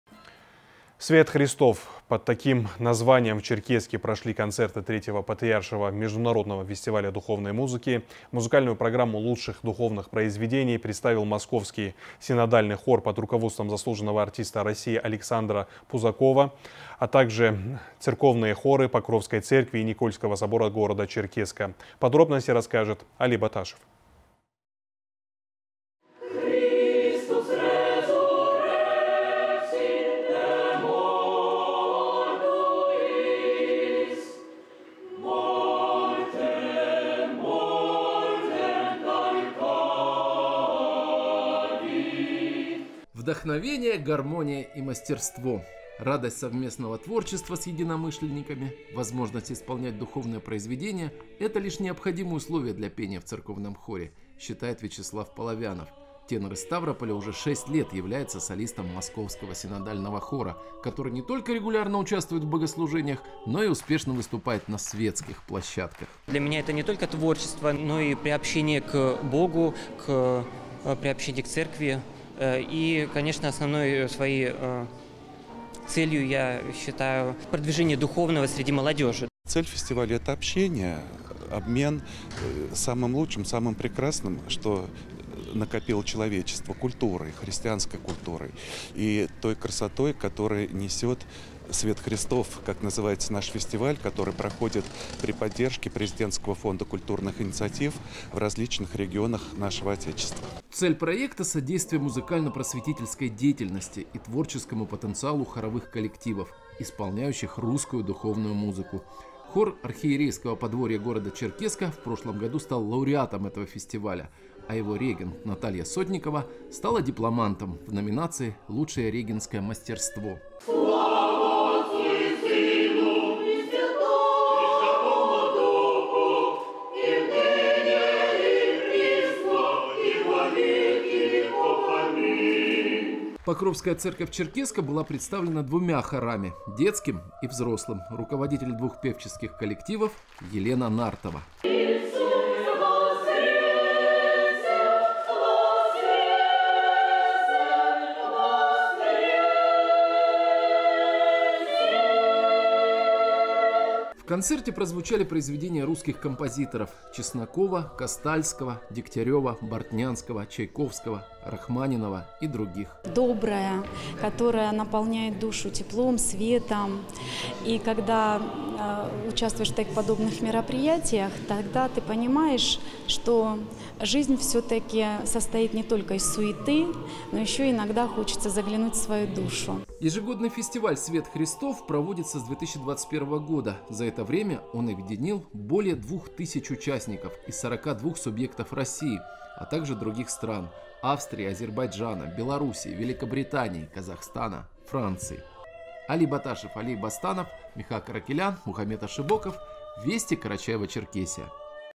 В Черкесске прошли концерты духовной музыки фестиваля "Свет Христов"
Свет Христов. Под таким названием в Черкесске прошли концерты III Патриаршего международного фестиваля духовной музыки. Музыкальную программу лучших духовных произведений представил Московский синодальный хор под руководством заслуженного артиста России Александра Пузакова, а также церковные хоры Покровской церкви и Никольского собора города Черкесска.